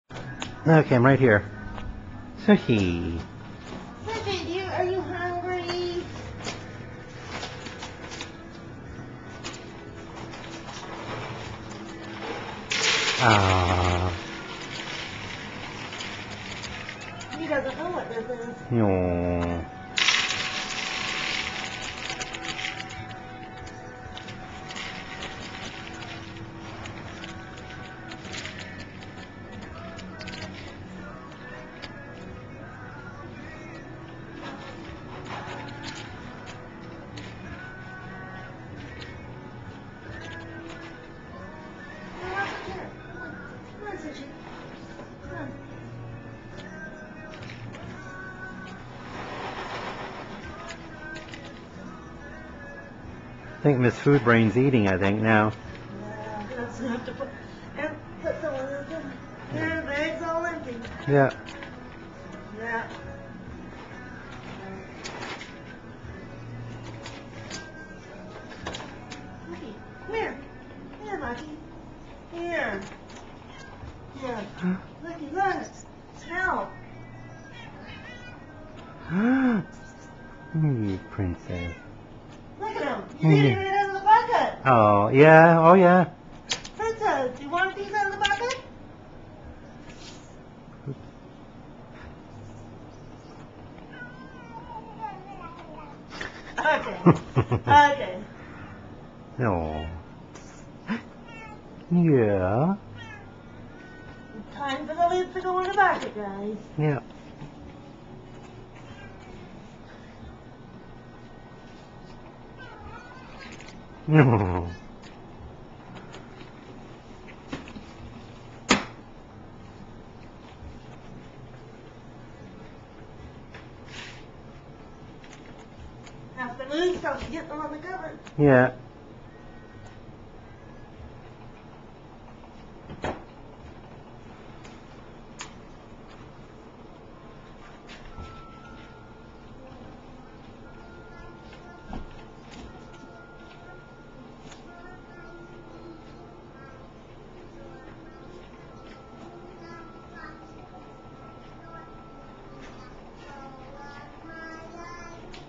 Filling cat food containers
My girl friend's filling up the containers where we keep the cat food in.  Lucky and Princess, are near by wanting the food and they both eat a couple bites from the open container.